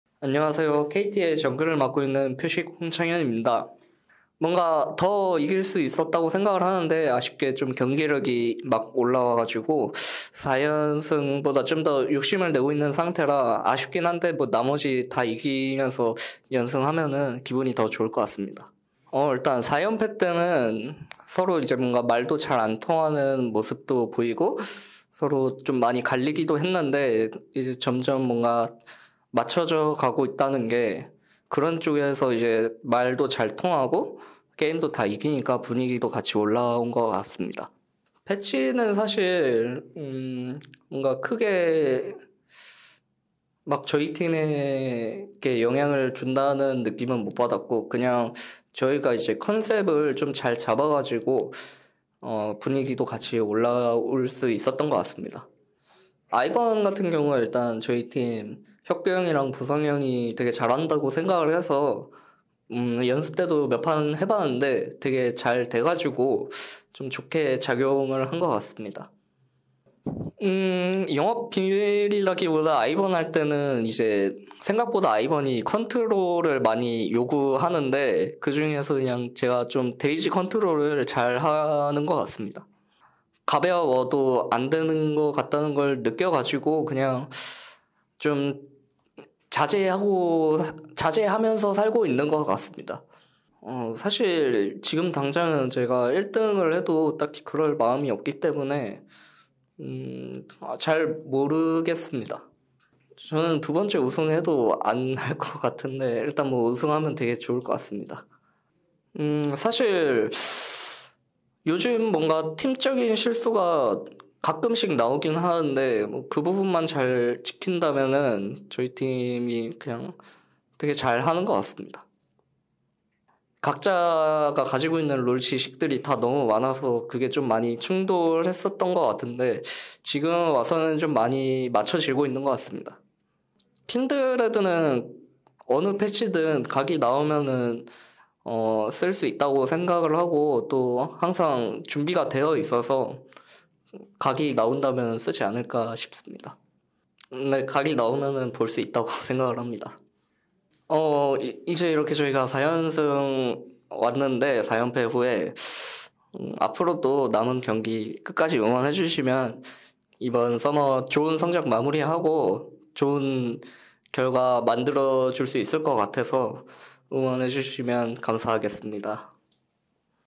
[LCK 현장 인터뷰] KT 표식 "각 나오면 DRX전 킨드레드 꺼낼 수 있다"
2024년 7월 12일 lck 승리팀 인터뷰 kt 롤스터 '표식' 홍창현